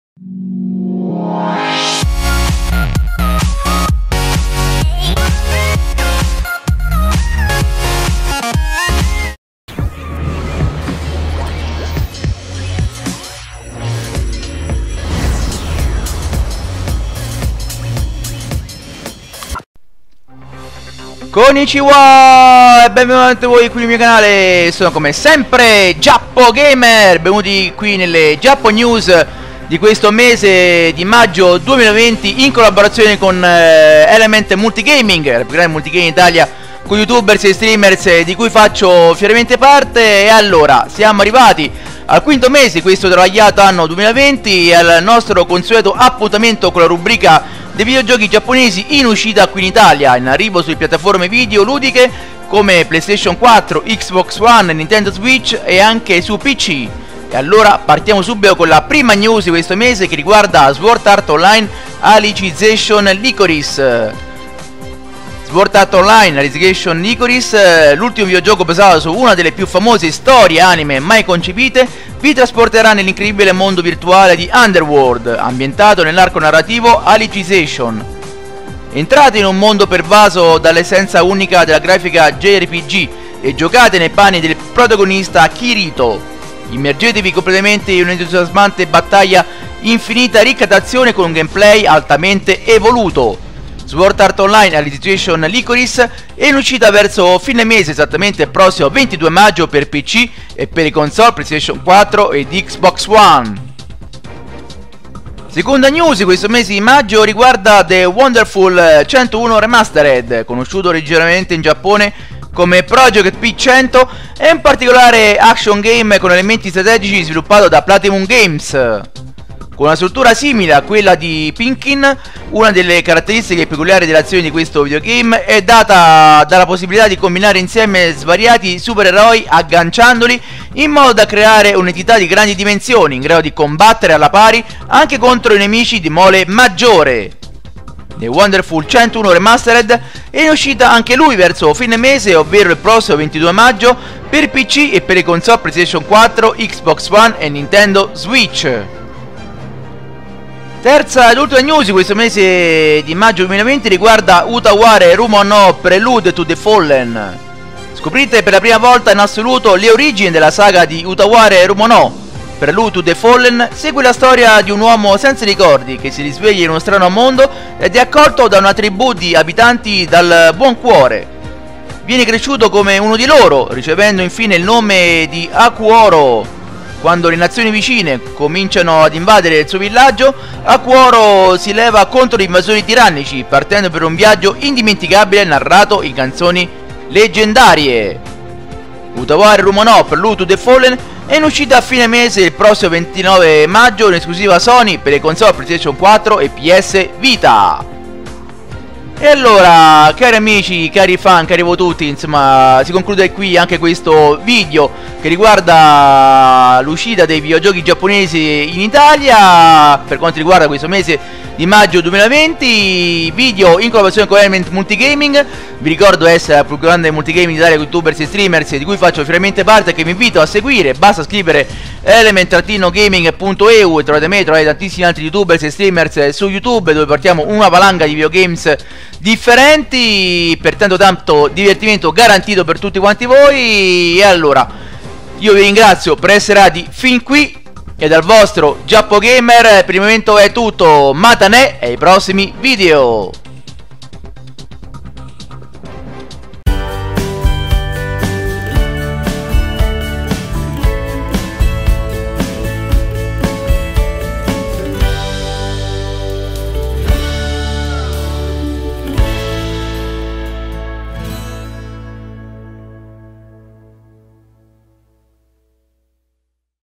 GGN | Maggio 2020
★ Vi ringrazio per i Likes e per le condivisioni ★

Konnichiwa a tutti Voi!!!
Eccoci arrivati al quinto appuntamento dell'anno con le GAMING NEWS per quanto riguarda le uscite dei videogames giapponesi in questo mese di Maggio 2020!!! Avete in mente di comprare qualcosa? Buona visione e mi racocmando commentate e condividete yattaaaaaaaaaaaa ^_^